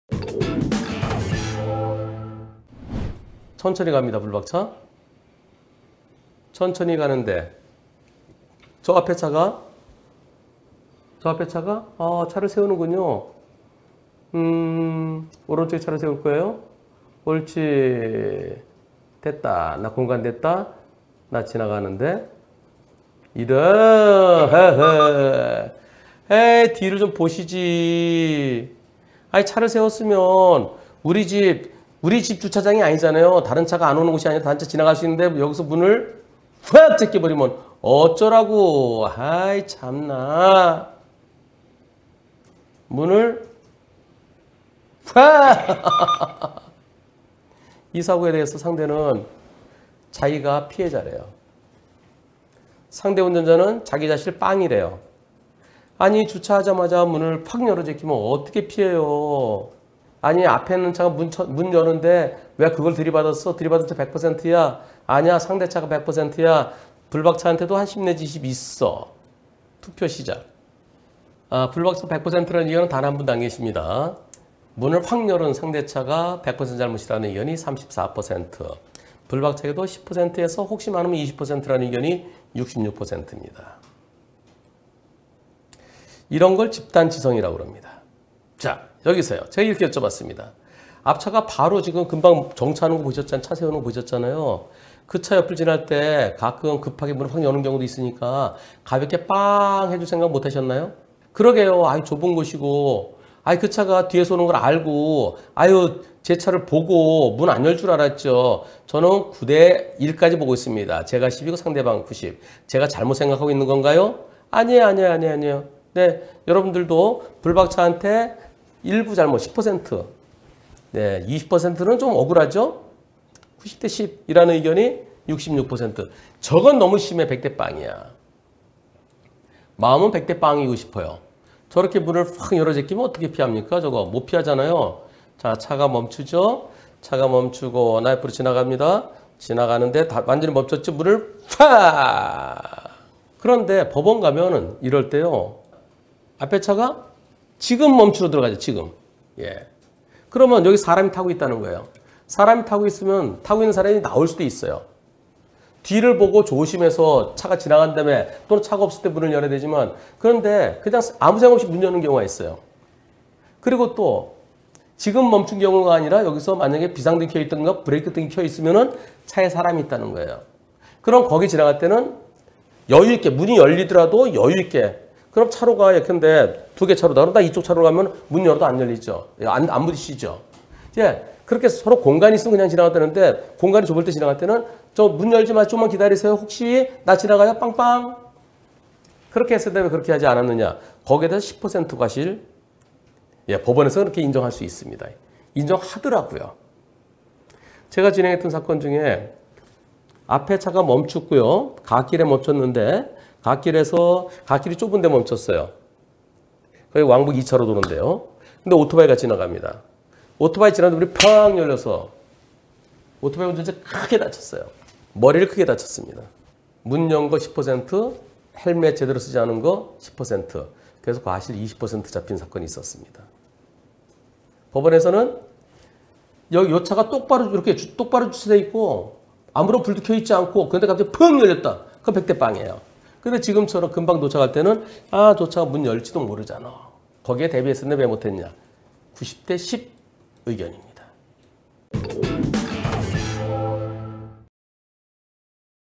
14791회. 방금 정차한 차 옆을 막 지나는데 문이 확 열리면서 그만...
25968,
220214 (월) 오전 생방송
열리는 문을 조심하라!!

산업유통단지내 건물과 건물 사이 도로.

우측 노상 주차장에 차를 세운 앞 승용차가
블박차가 막 지나갈 때 문을 확 열어서 사고

상대는 본인 이 피해자면서, 과실 0 이라고 주장하는데...

상대방 운전자가 본인 과실 0으로 주장하는 상황이라 너무 황당해서 문의 드립니다.

주차하자마자 문을 확!~ 열어버리는데 급정거 할 틈도 없었네요.

* 투표 *

1. 블박차 100% 잘못

2. 상대차 100% 잘못 (34%)

3. 블박차에게도 10~20% (66%)

한문철 변호사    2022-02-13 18:37
앞차가 바로 정차하는 걸 보셨기에

그 차 옆을 지나기 전에 가볍게 빠앙~ 해 줄 생각은 못하셨는지요? ㅠㅠ

질문자는 몇 대 몇 의견이신가요?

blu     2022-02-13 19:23
그러게요... 좁은 곳이고 후방카메라도 있을텐데 후방에서 차가 온다는 걸 인지하신줄 알았는데 이렇게 되어 버렸네요...

90:10까지는 보고 있습니다... 제가 10, 상대방 90

제가 잘못 생각하고 있는건지요?...

* 의견

앞에 방금 정차한 차는 언제든지 갑자기 문을 열 수 있으니

좁은 공간으로 앞차를 지날 땐

속도 줄이고

가볍게 빠앙~ 해 주는 게 안전

마음은 100:0 이지만

법원가면,
사람이 타고 있기에 타고 있는 사람이 나올 수 있기에 조심했어야 한다는 점에서

블박차에도 일부 과실(10%) 인정하는 추세

유사사건 소송 결과
오토바이가 개문사고로 크게 다쳤을 때

문 열림에 대비못한 과실 10%
헬멧 미착용 10% 판결.

이번 사고는 90:10 의견

쭌,